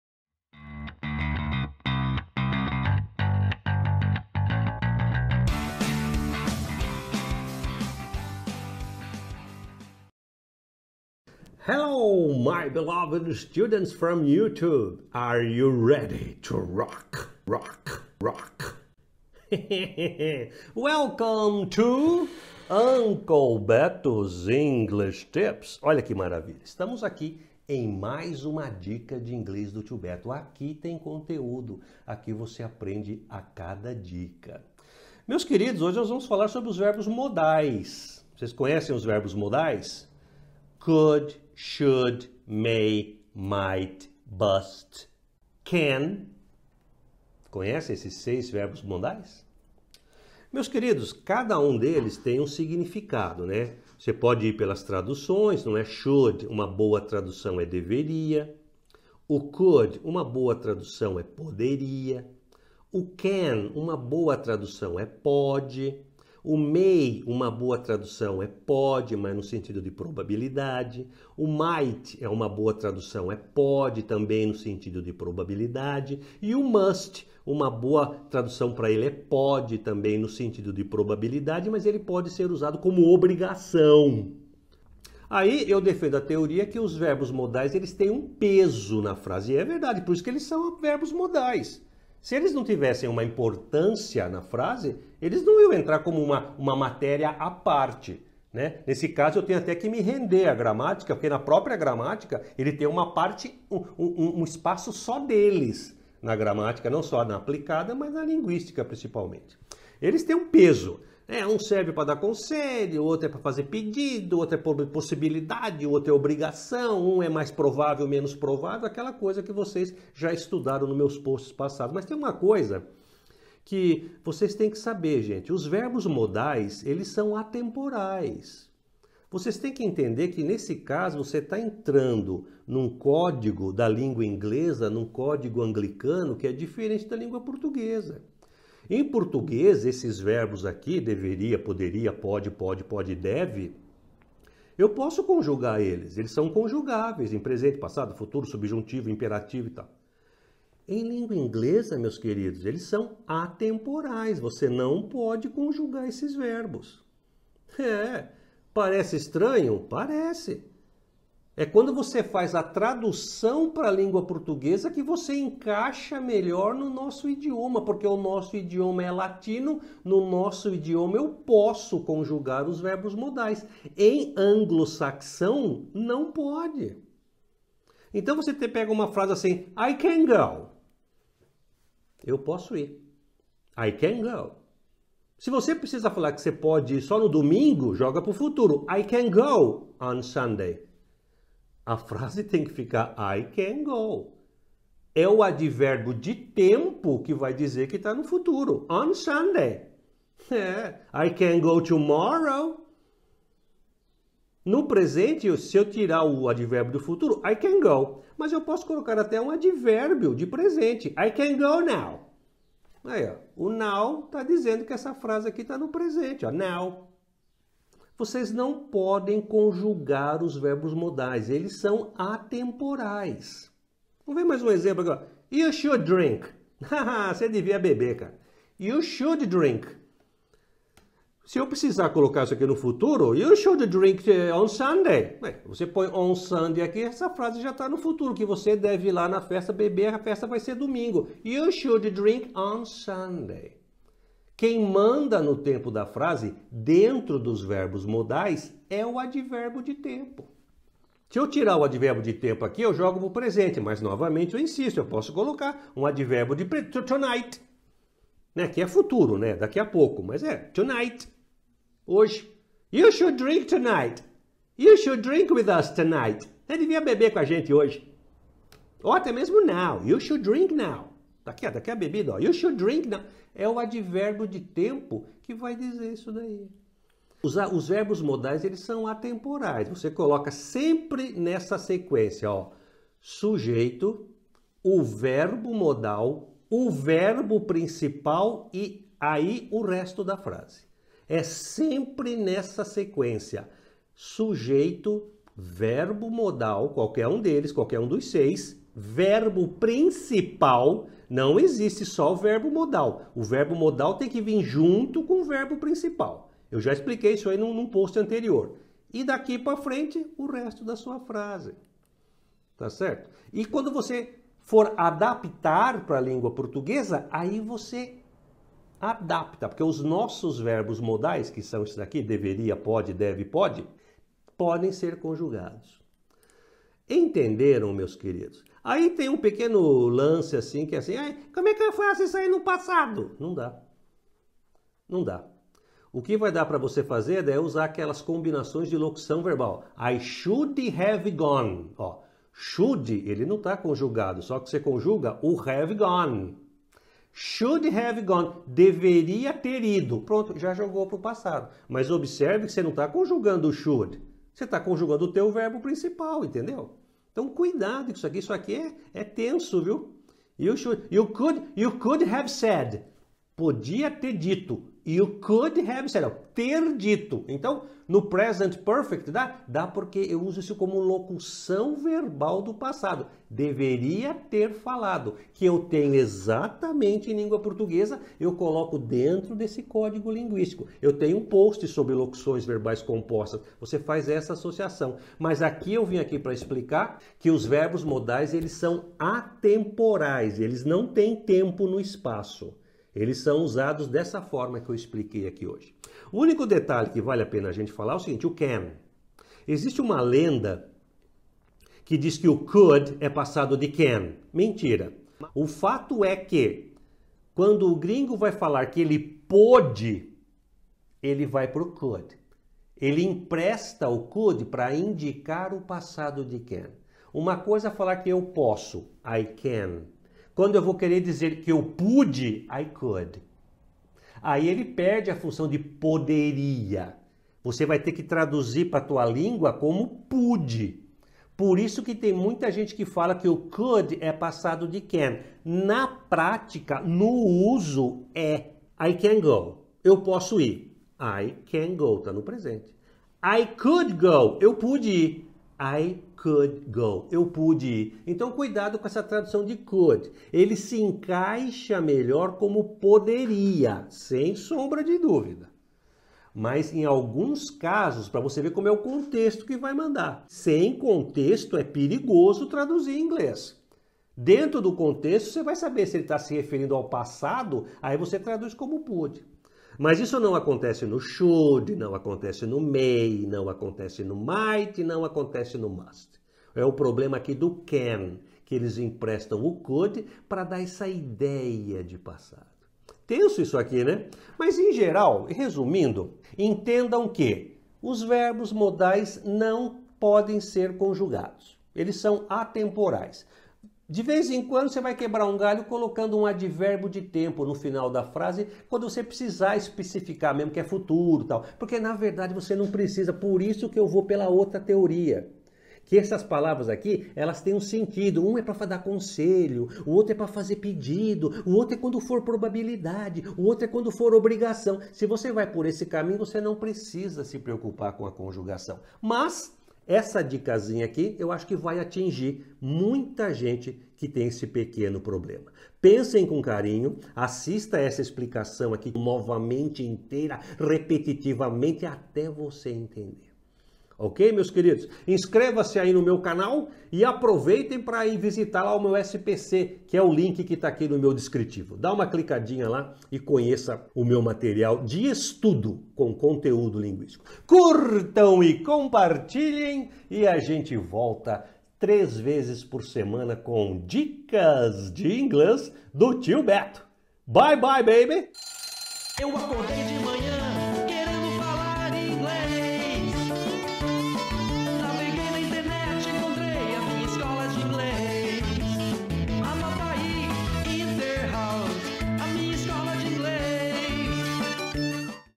Verbos Modais são ATEMPORAIS - Dica de Inglês do Tio Beto
SPC - Só Para Conversar
O material de conteúdo de inglês do Professor Beto Camargo

Até rimou, essa informação é fundamental, verbos modais são atemporais, não são conjugados em nenhum tempo verbal